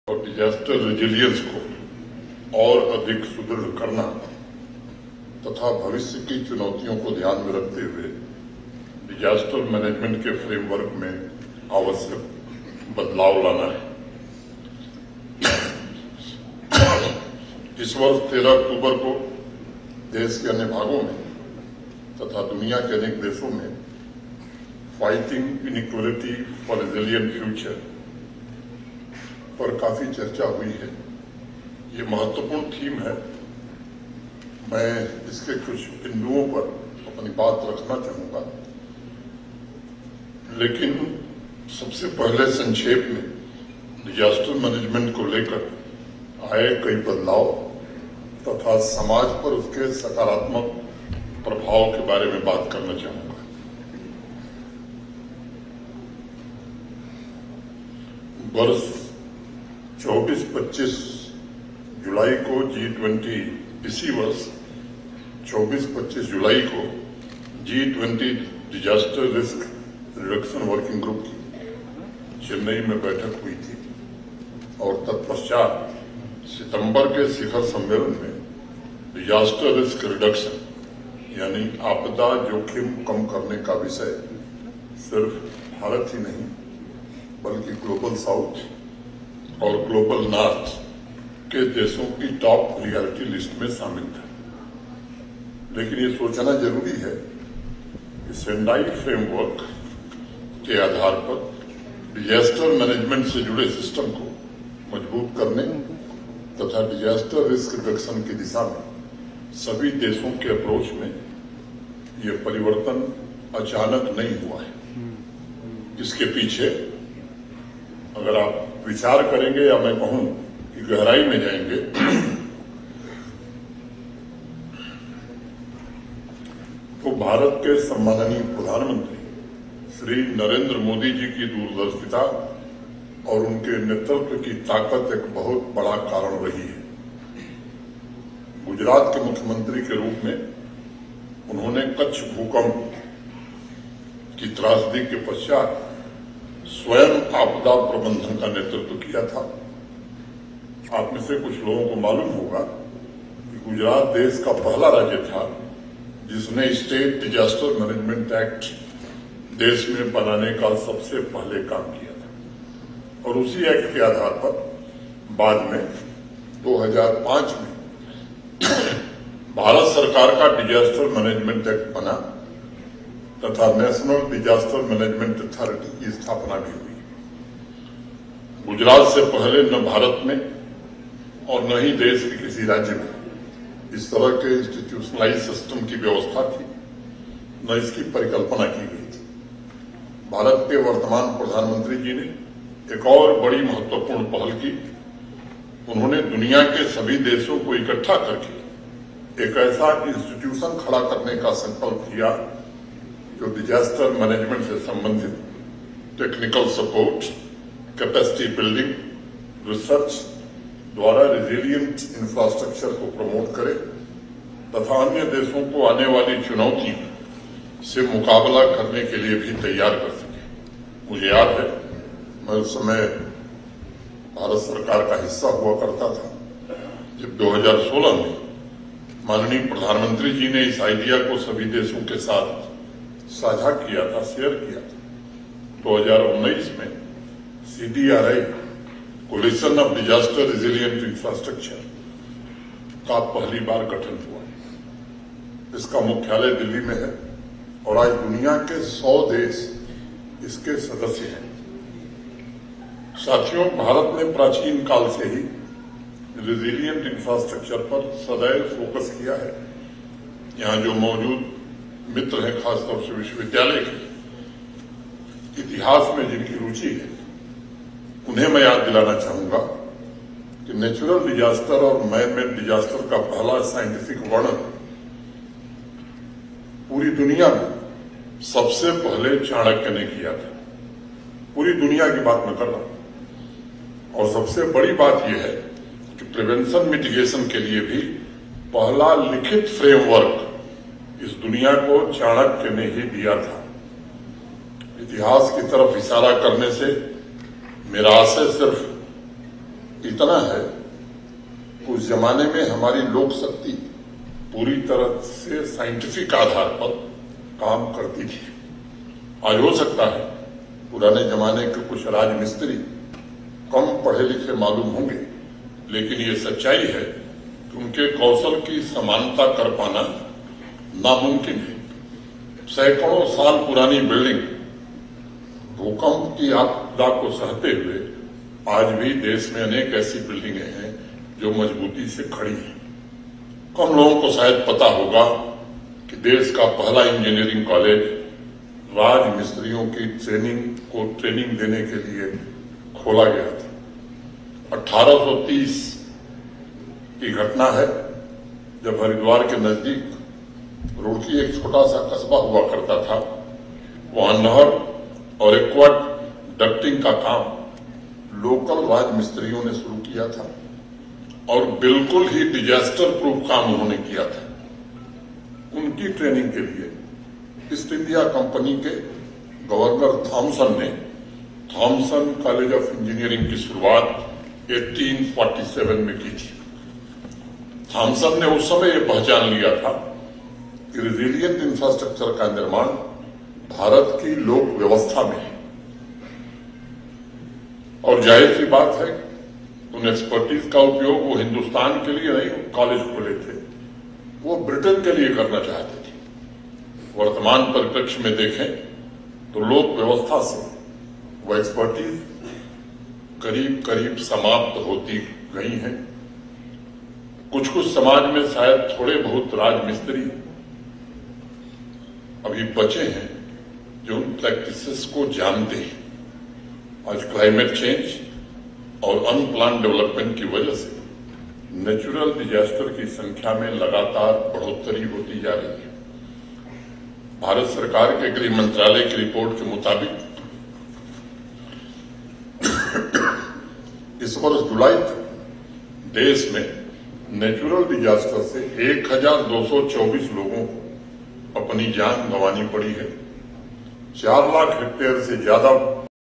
LG Manoj Sinha addressed the program at the Convocation Center of Kashmir University | News18 JKLH
J&K | LG Manoj Sinha addressed the program at the Convocation Center of Kashmir University. The theme of the program is 'Fighting inequality for the future'...